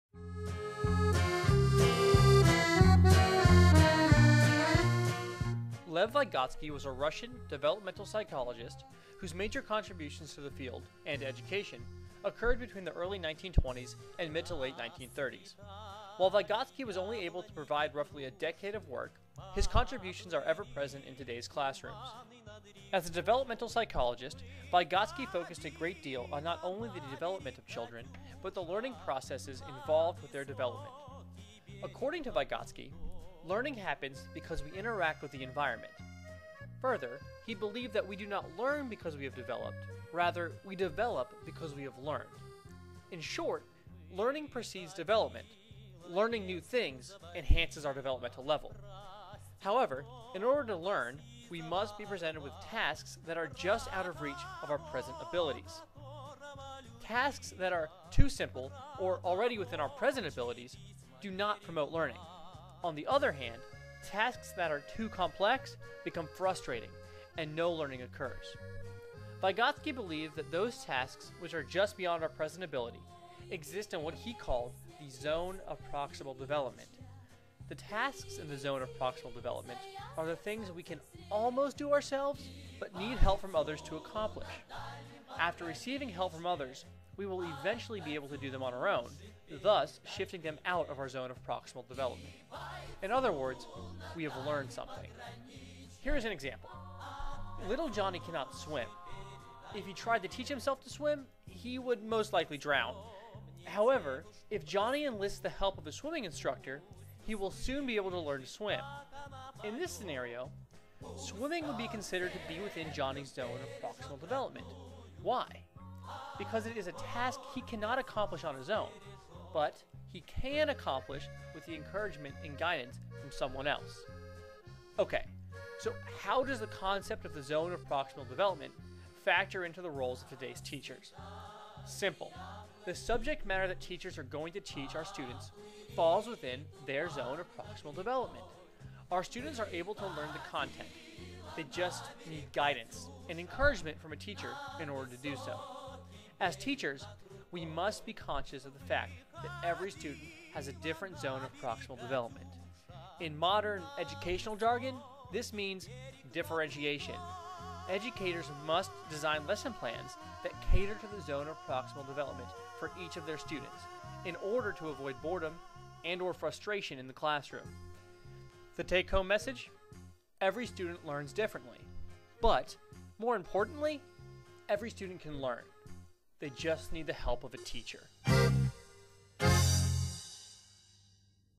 Vygotsky's Zone of Proximal Development
What is the ZPD and how does it relate to teaching?

UPDATE: Sorry about the music folks! I have upload a version without the music.Link at the beginning of the video
I have received requests for more videos. Please let me know specific topics and I would be happy to post. Thanks!